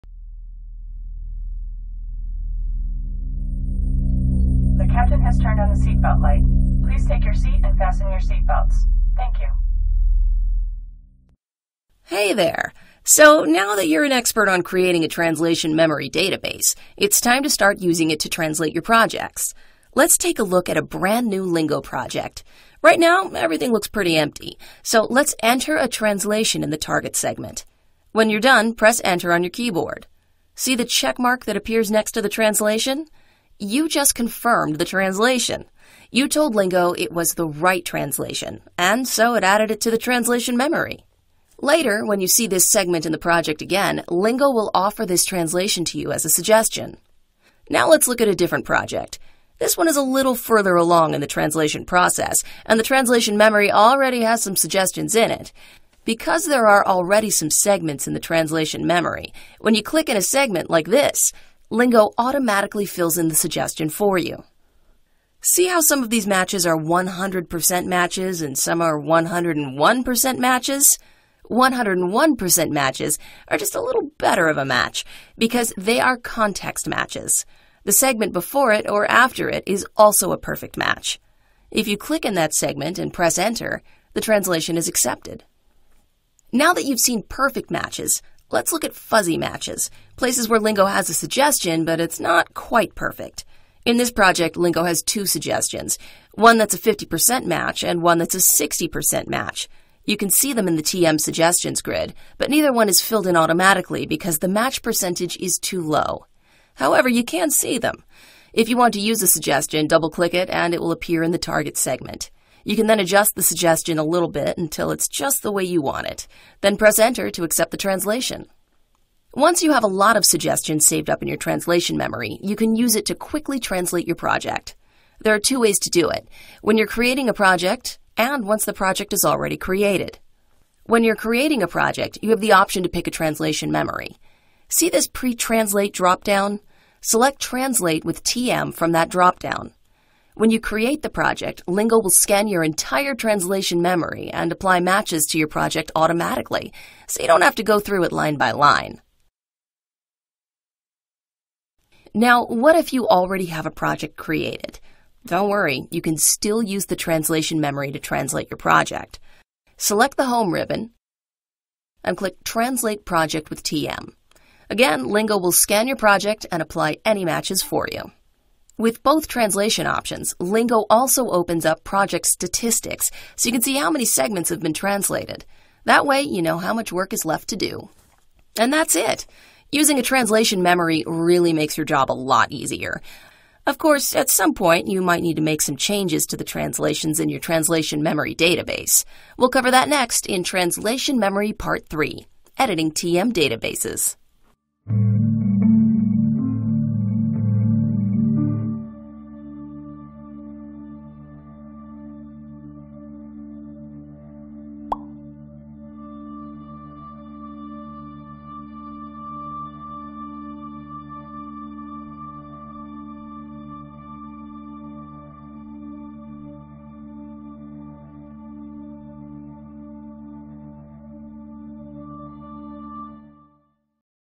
Translation Memory Part 2: Using TM
This tutorial shows you how to make use of translation memory suggestions in a MadCap Lingo project. It also discusses matching percentages.